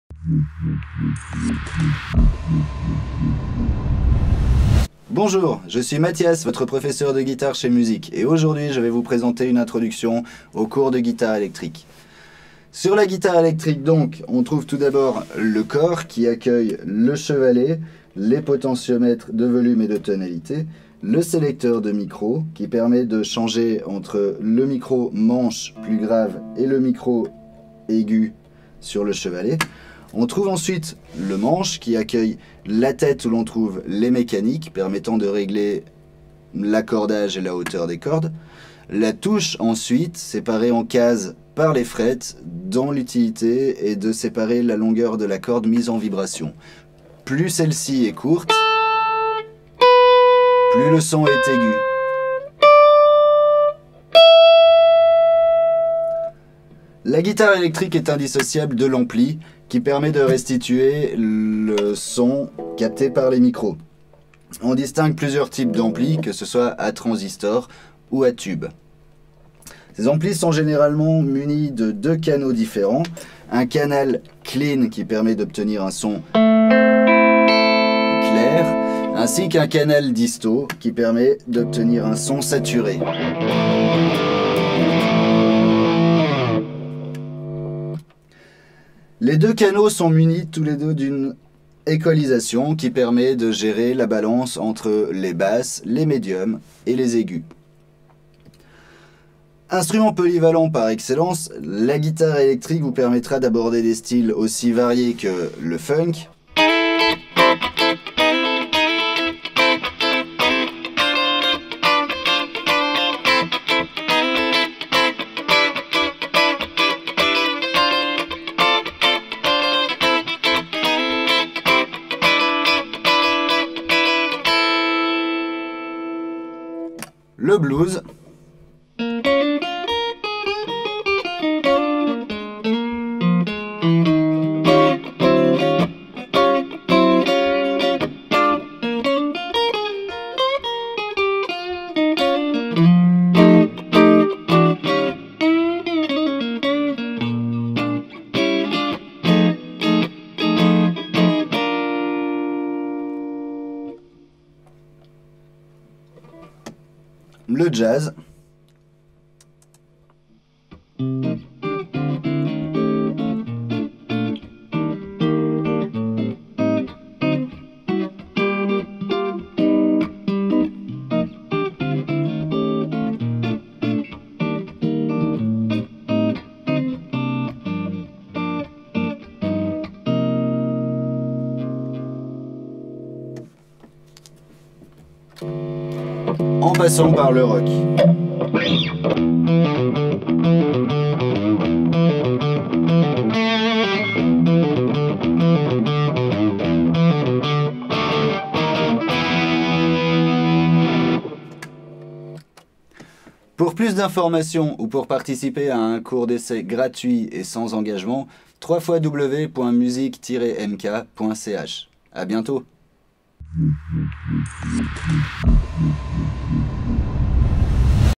Guitare Electrique
Un instrument moderne et offrant une large palette de sons. Quel que soit votre genre musical la guitare électrique vous permettra d’évoluer à travers toutes les musiques actuelles.